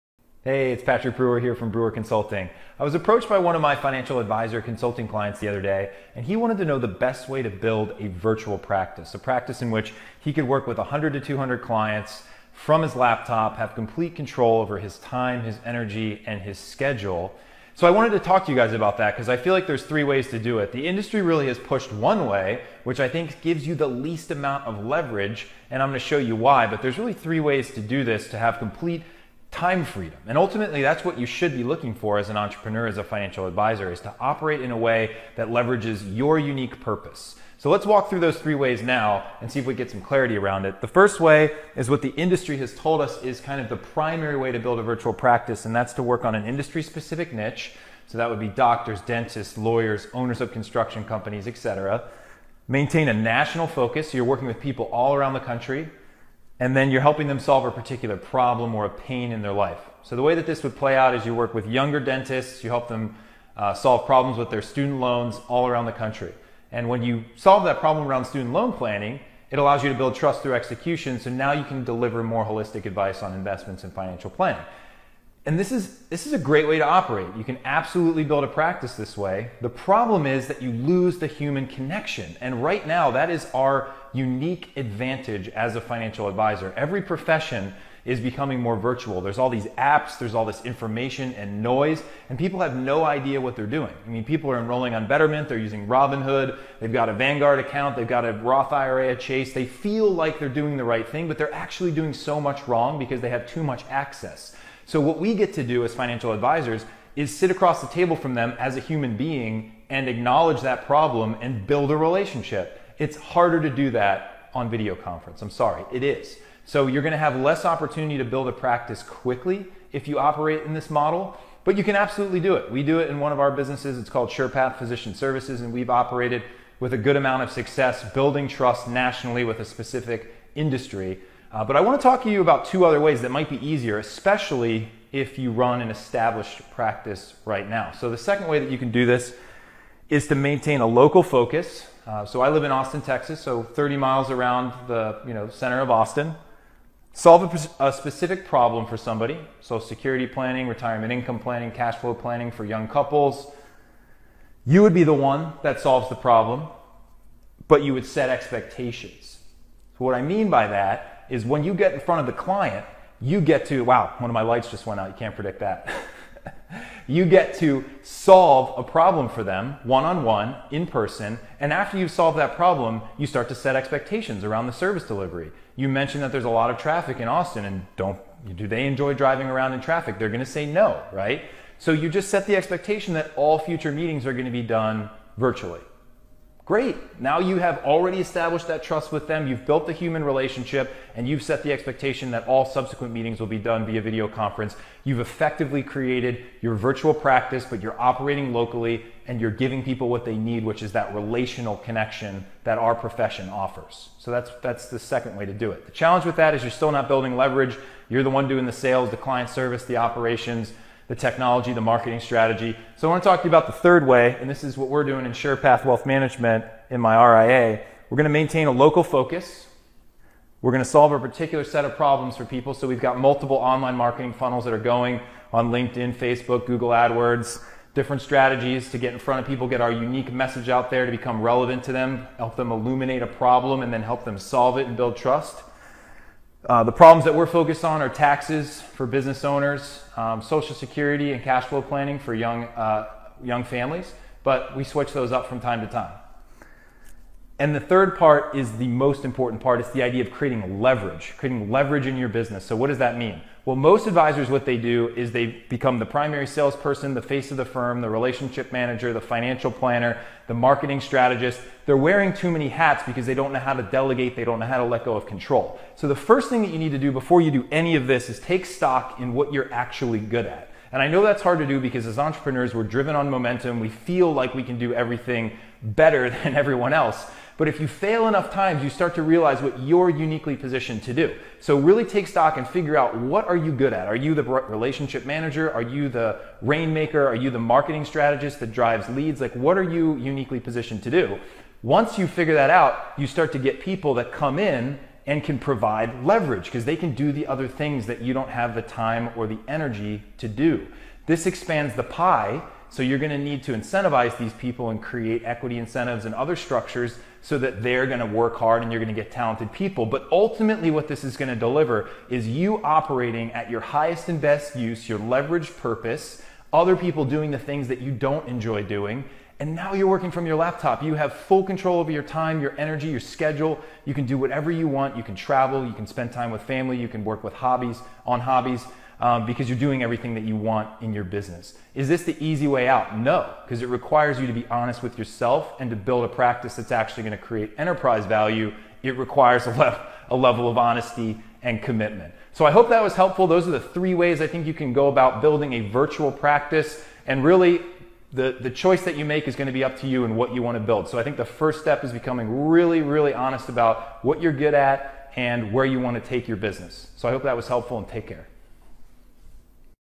Financial Advisor Marketing: How to Build a "Virtual" Advisory Practice
There are 3 ways to build a “virtual” advisory practice…

#2 and #3 (most effective) may surprise you.

Get your FREE copy of The Definitive Guide to an Effective Online Marketing Strategy for Financial Advisory Practices